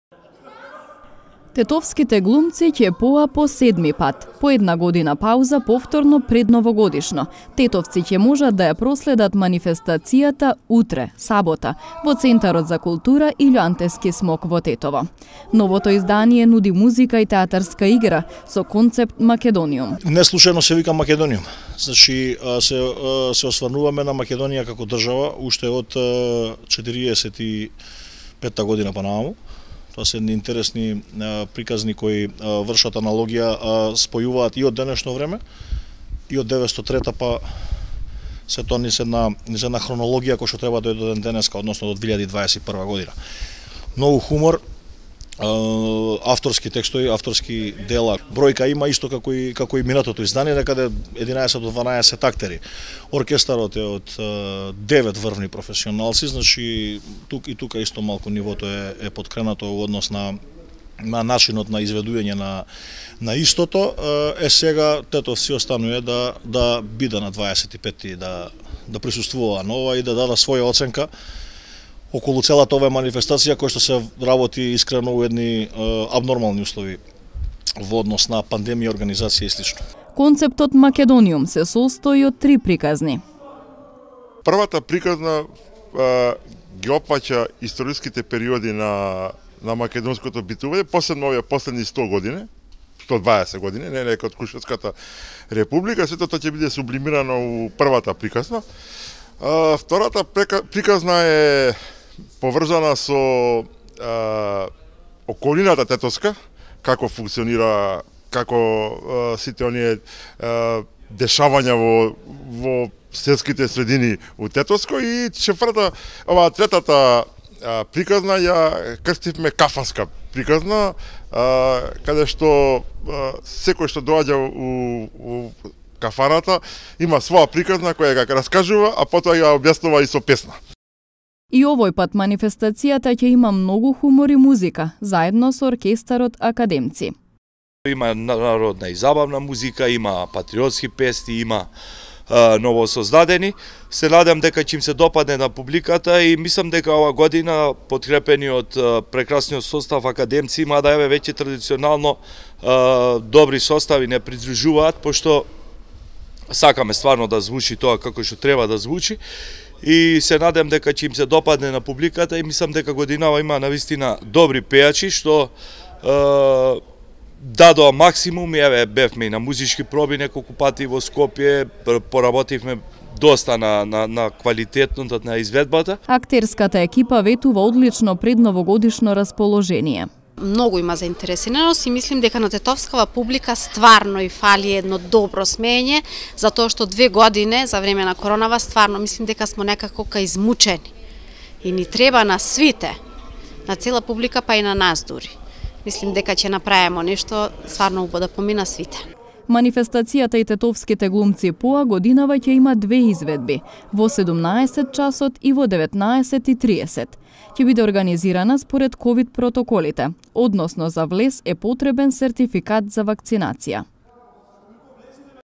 Во сабота „И тетовските глумци поа“
7-то издание на манифестацијата „И тетовските глумци поа“ ќе се одржи во сабота во Центарот за култура во Тетово. Оваа година ќе има две изведби во еден ден.